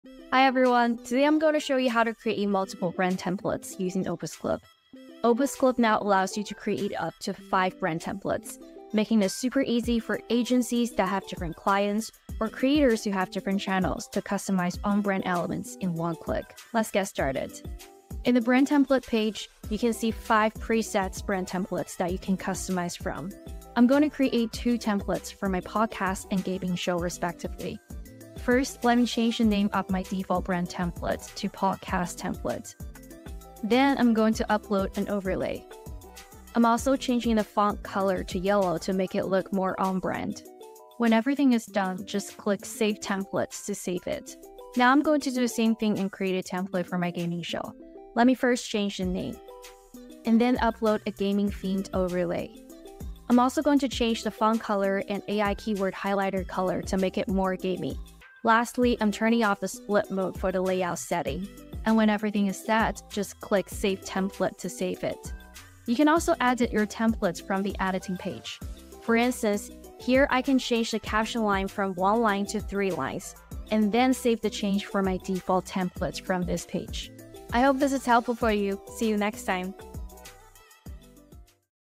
How To Create a Stunning Custom Brand Templates with opusclip in Minutes | 2024
I couldn't find the specific video titled "Create Stunning Custom Brand Templates with OpusClip in Minutes!" However, I found information about OpusClip's capabilities that are likely to be featured in such a video.

OpusClip is a tool designed for creating short, impactful videos for brand promotion and marketing. It allows users to make on-brand short videos by using custom brand templates and uploading logos and calls-to-action (CTAs), enhancing brand awareness​​. The tool is beneficial for generating more leads and exposure, as posting shorts on platforms like LinkedIn, YouTube Shorts, TikTok, Instagram, and Twitter can significantly extend audience reach​​.

Additionally, OpusClip helps save time and money by efficiently converting long-form content into multiple short videos. For instance, it can transform an hour of long-form content into 25 on-brand shorts in under 10 minutes, streamlining content creation and distribution​​. The technology behind OpusClip leverages big data to analyze video content in relation to current social and marketing trends, helping to create engaging and coherent short videos with dynamic captions, AI-relayout, and smooth transitions, concluding with a strong call-to-action​​.